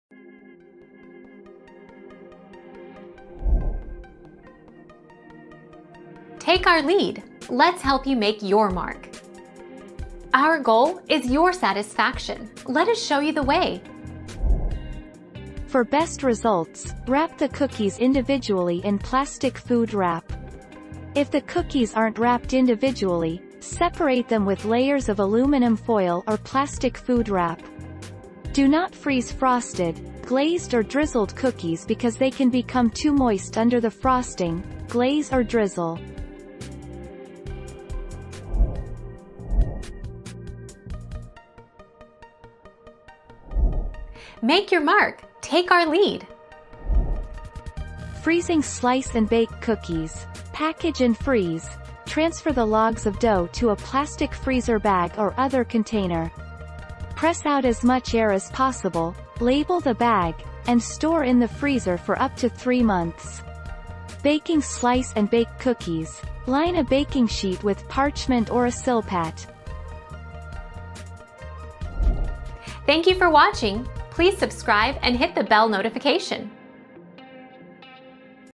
What is the best way to freeze cookies?
00:00 - What is the best way to freeze cookies?
00:40 - Can you freeze cookie dough and bake later?

Laura S. Harris (2021, January 13.) What is the best way to freeze cookies?
    AskAbout.video/articles/What-is-the-best-way-to-freeze-cookies-210338